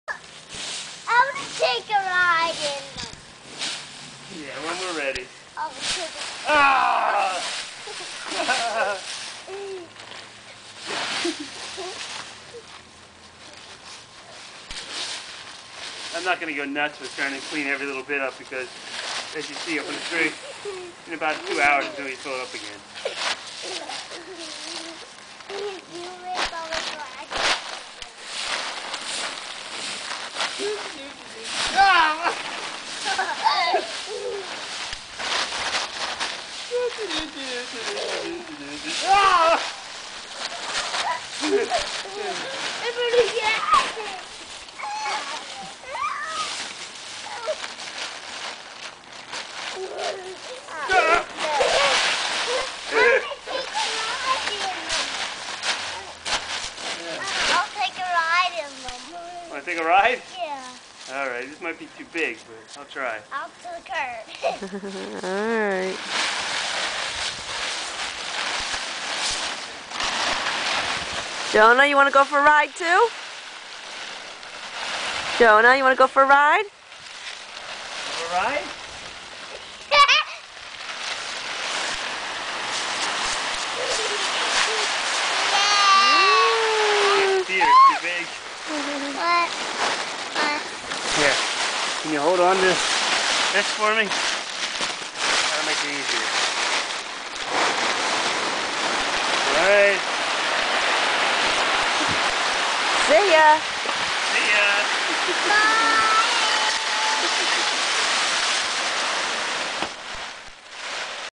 Noah and Jonah Leaves 11 07
Noah and Jonah play in leaves.